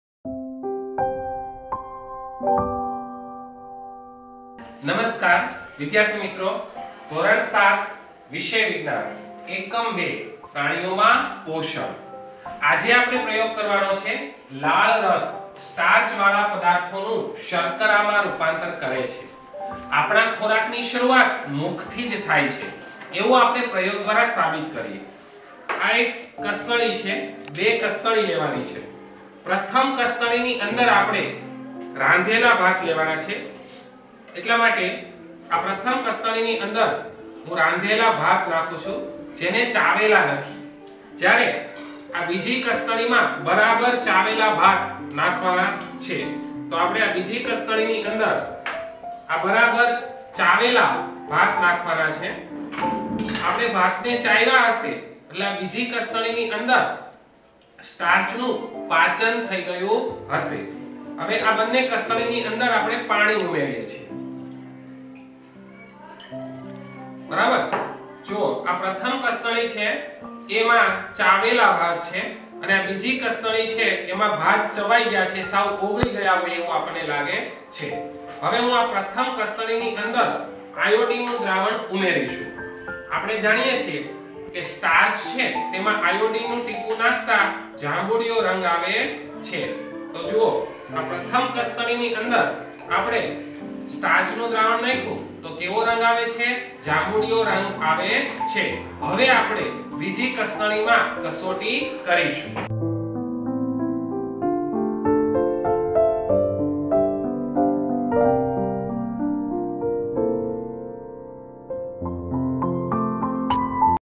ધો-7વિજ્ઞાન,L.2 પ્રાણીઓમાં પોષણ,પ્રયોગ–લાળરસ સ્ટાર્ચવાળા પદાર્થોનું શર્કરામાં રૂપાંતર કરે છે.patrt-1
શ્રી કોઠાપીપરીયા પ્રાથમિક શાળા
 Sandipsir Dhaduk